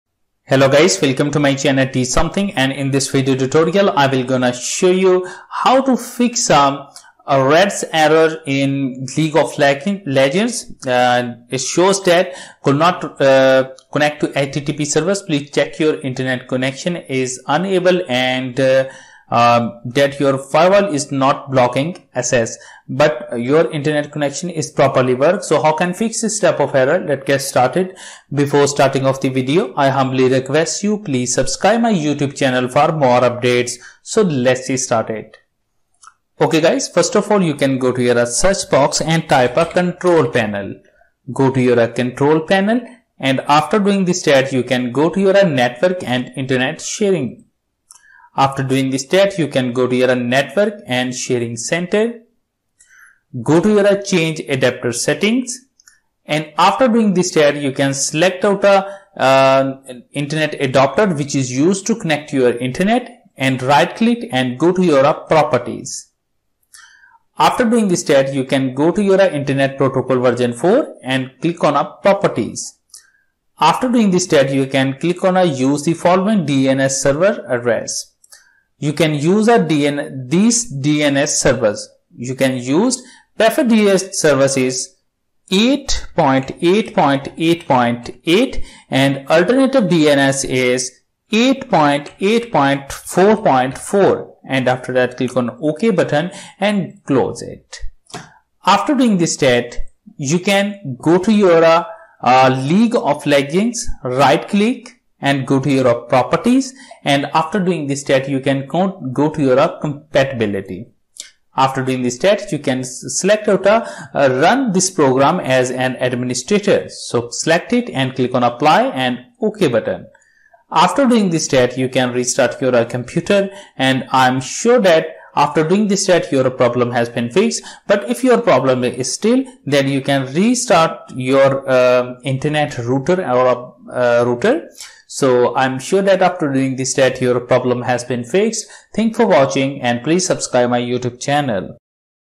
How to Fix League of Legends RADS ERROR in Windows 10
In this video tutorial i will gonna show you How to Fix League of Legends RADS ERROR in Windows 10.